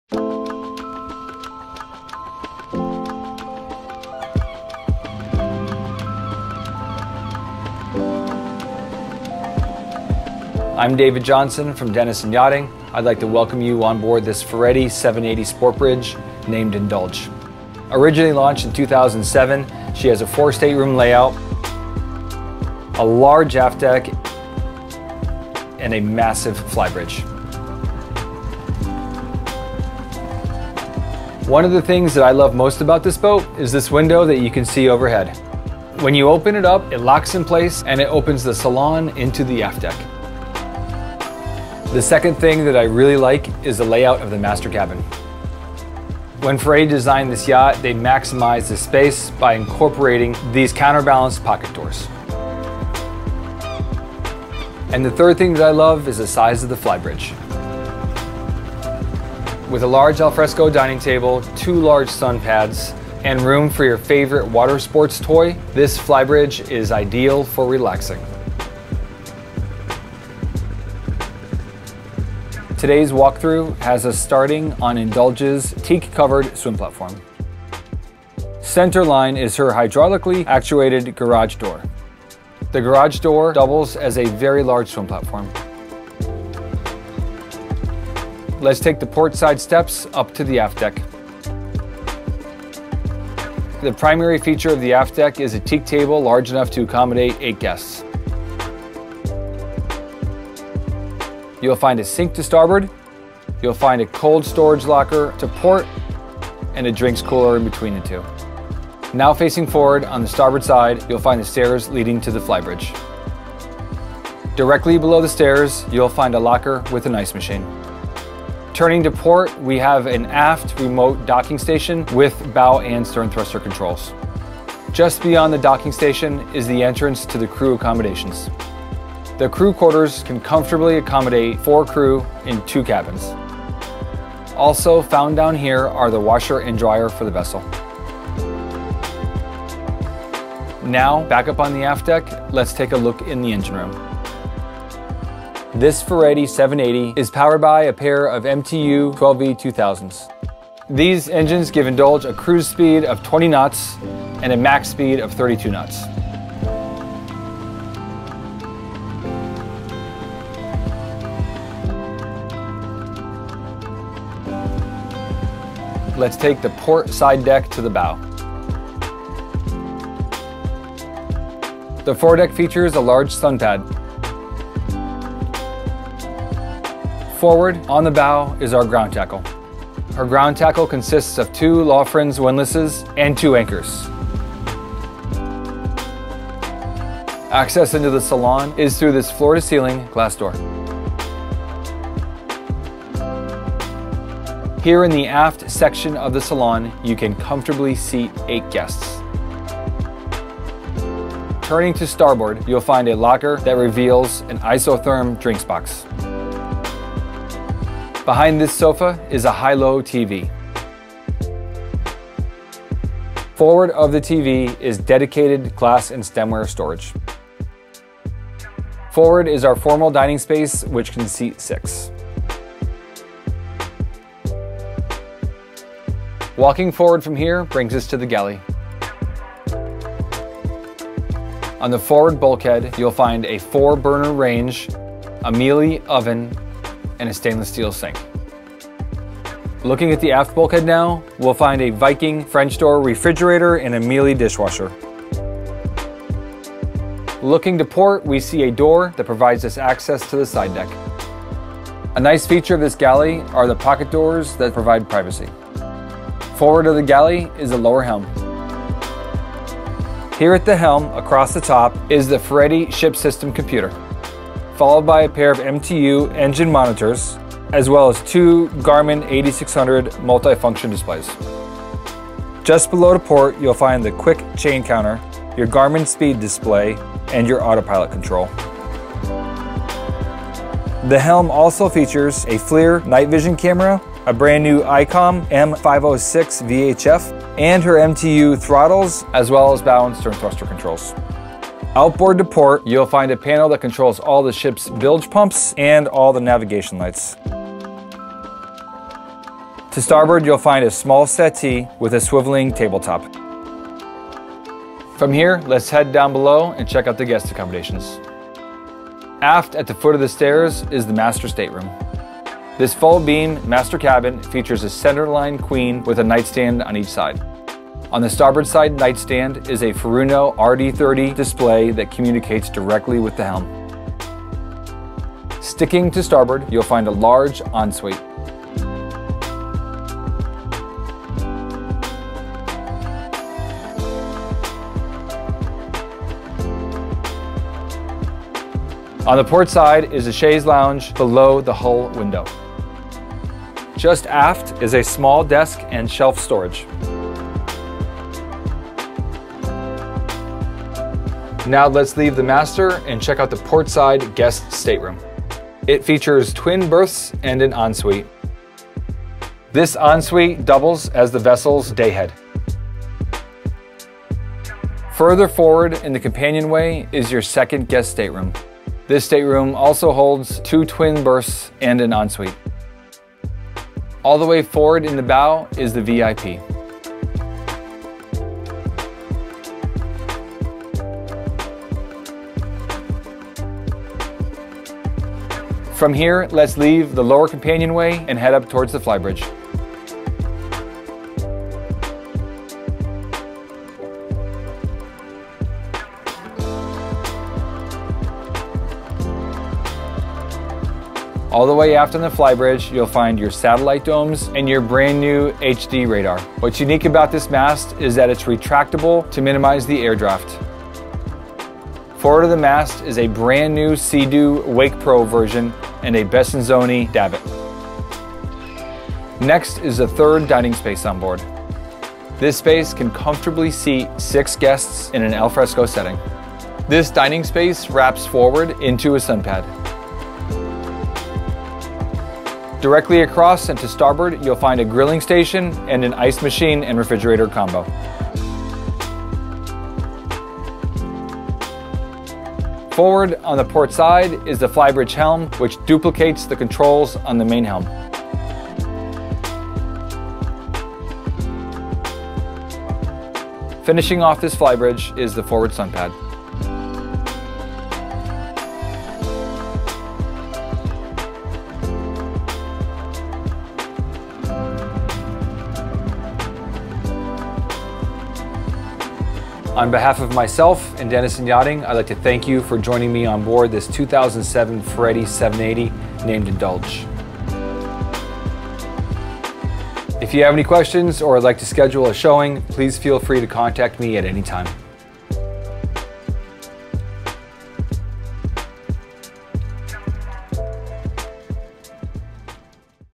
78 Ferretti Motoryacht Walkthrough [$1,299,000]
78' Ferretti Yachts 2007 [INDULGE]

INDULGE was meticulously cared for by her original owner and is still operated that way now, with an open checkbook policy.

Her radar arch was removed and replaced with a mast that can be lowered to allow the vessel to pass under low lying fixed bridges. This modification makes this one of the only 80' yachts that can complete the Great Loop and many other hard to access locations.

INDULGE has a 4 guest stateroom layout that includes a large full beam Master Cabin with huge hull side windows. Aft of the engine room are the two crew cabins. Each crew cabin has two bunks. Her interior is in excellent condition and still shows like a new yacht.

A very nice option on the INDULGE is the huge swim platform and garage. When the transom door is opened the garage is exposed and the swim platform is lowered to the water.

0:00 Introduction
1:16 Swim Platform
1:30 Aft Deck
2:09 Crew Accommodations
2:23 Engine Room
2:48 Foredeck
3:10 Salon
3:47 Galley
4:16 Lower Helm
5:10 Master Stateroom
5:53 Guest Staterooms
6:34 Flybridge + Upper Helm
7:50 Outro